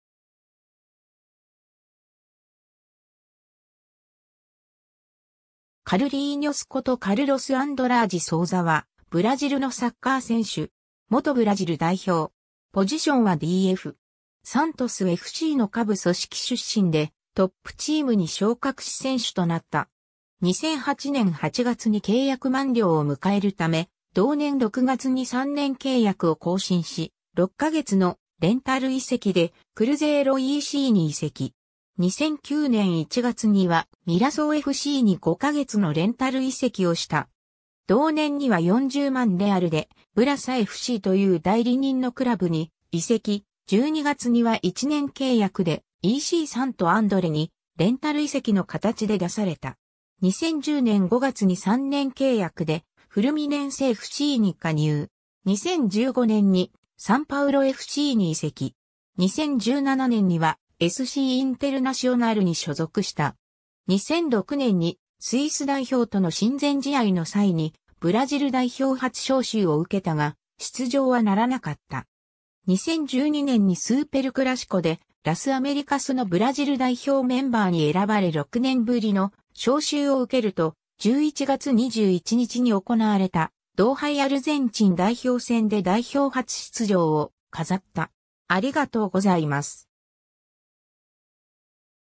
カルロス・アンドラージ・ソウザ
ECサント・アンドレの選手
カルリーニョス（Carlinhos）ことカルロス・アンドラージ・ソウザ（ポルトガル語: Carlos Andrade Souza、1987年1月23日 - ）は、ブラジルのサッカー選手。
元ブラジル代表。
 サントスFCの下部組織出身でトップチームに昇格し選手となった。
2008年8月に契約満了を迎えるため、同年6月に3年契約を更新し、6ヶ月のレンタル移籍でクルゼイロECに移籍。
2009年1月にはミラソウFCに5ヶ月のレンタル移籍をした。
同年には40万レアルでブラサFCという代理人のクラブに移籍、12月には1年契約でECサント・アンドレにレンタル移籍の形で出された。
2010年5月に3年契約でフルミネンセFCに加入。
2015年にサンパウロFCに移籍。
2017年にはSCインテルナシオナルに所属した。
 2006年にスイス代表との親善試合の際にブラジル代表初招集を受けたが出場はならなかった。
2012年にスーペルクラシコ・デ・ラス・アメリカスのブラジル代表メンバーに選ばれ6年ぶりの招集を受けると、11月21日に行われた同杯アルゼンチン代表戦で代表初出場を飾った。